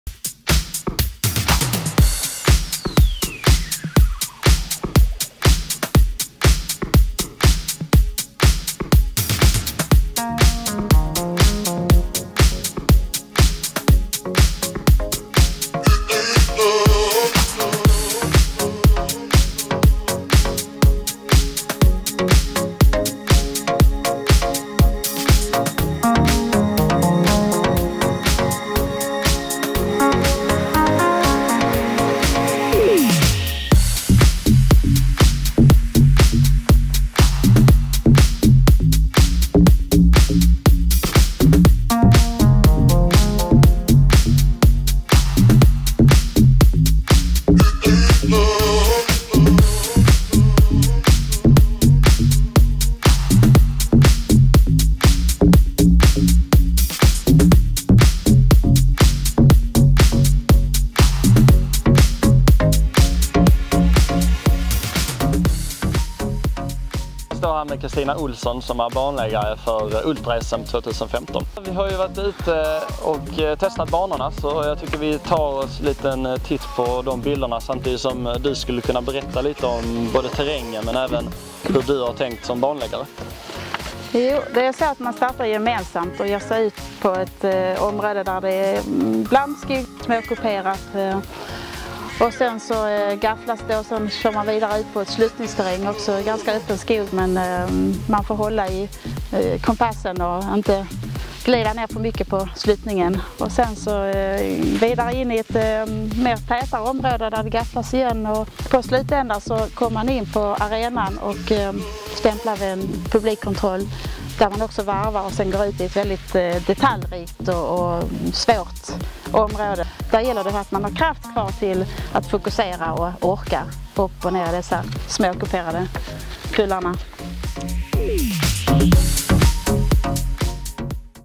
Orientering - Skåneslättens OL
Ultra-SM i Orientering 2015 går av stapeln i hjärtat av Skåne, nämligen på Söderåsen. Det kommer bjudas på tuff skånsk terräng bland ljuvlig bokskog och fina sluttningar.

Vi har varit ute och testat banorna inför nästa år och här är bilder från terrängen samt några ord från banläggare Christina Olsson.